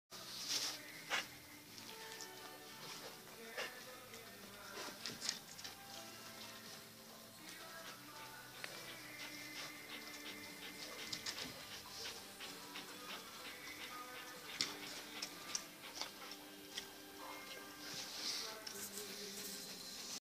Doodle Resort Dog's paradise 30 dec 2013
Guusje en Bubbels lekker stoeien